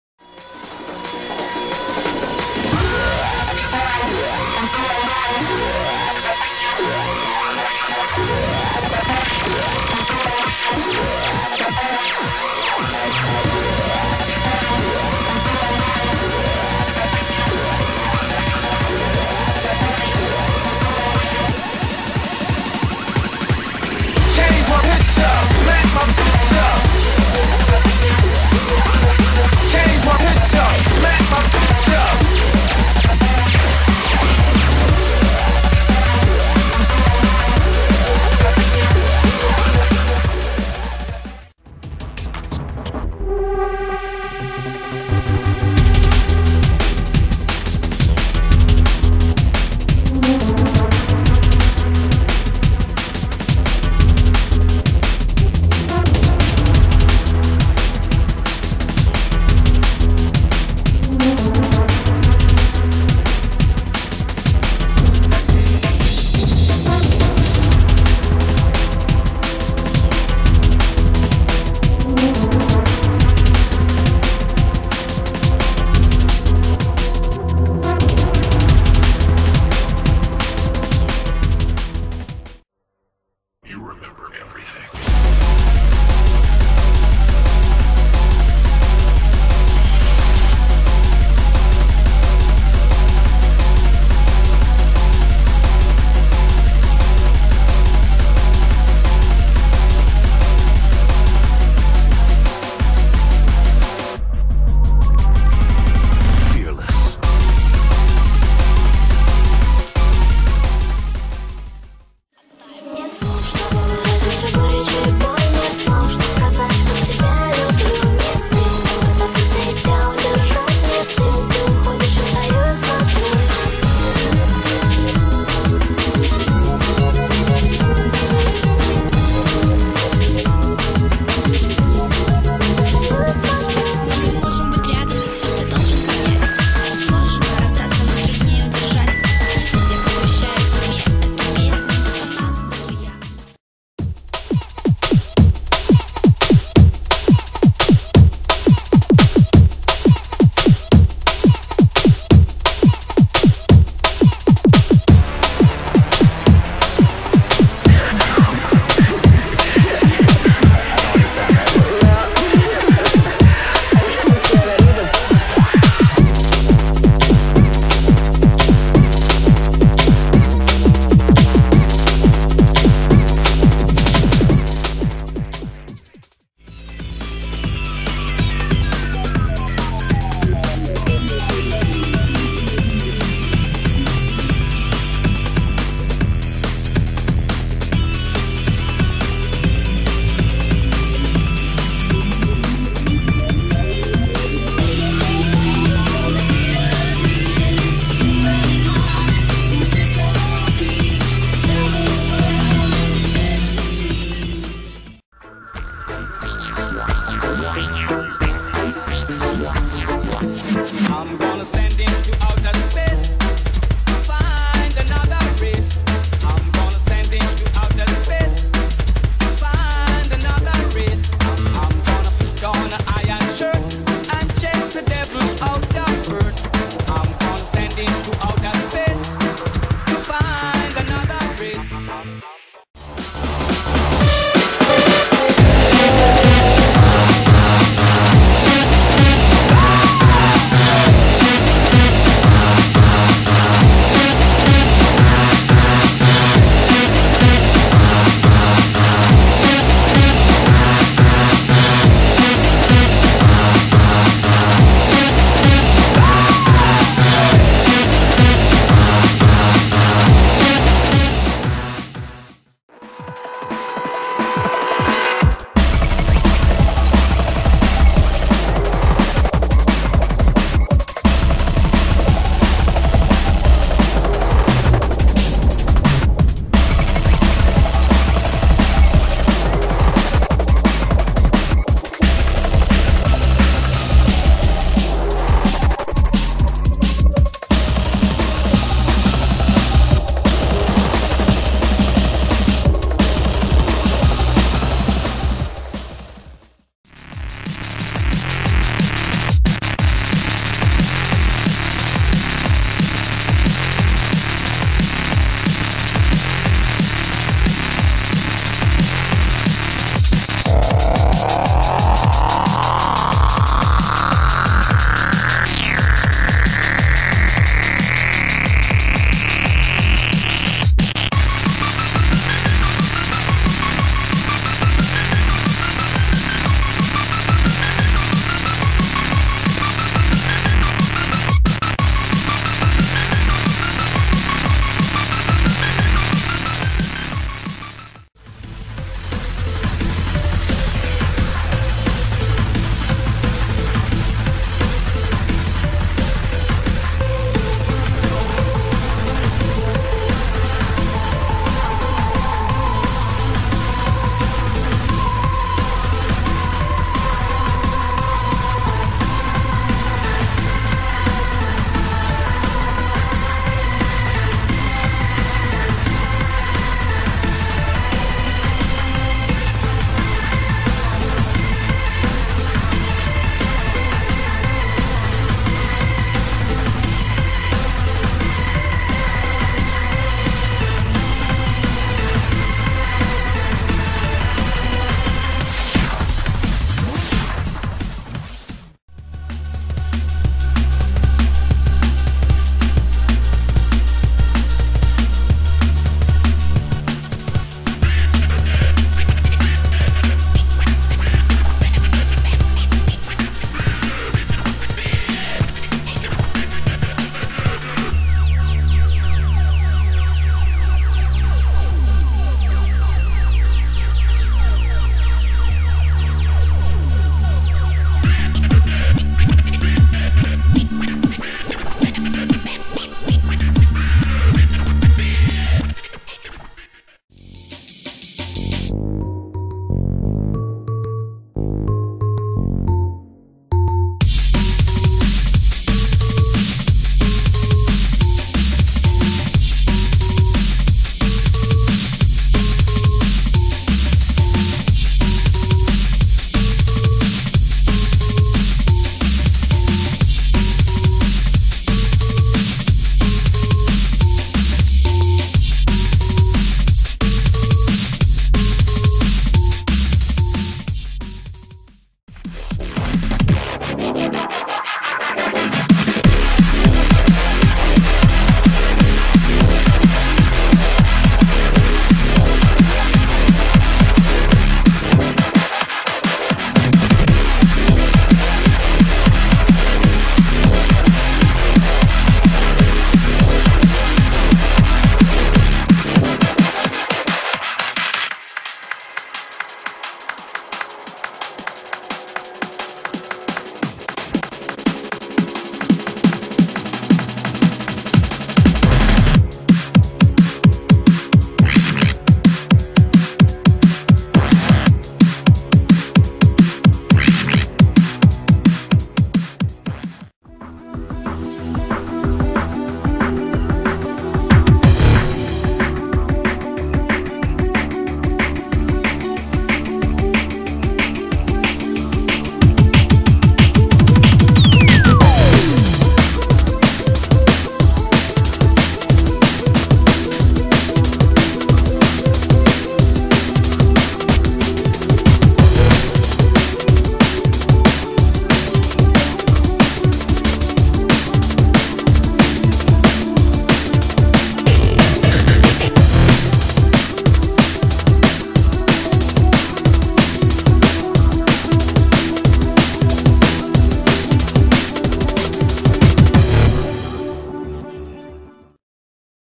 Top 15 DnB (2nd Part)
This is my new top 15 drum and bass songs, they're disordered.This recopilation include frenchcore, and hardtek artists.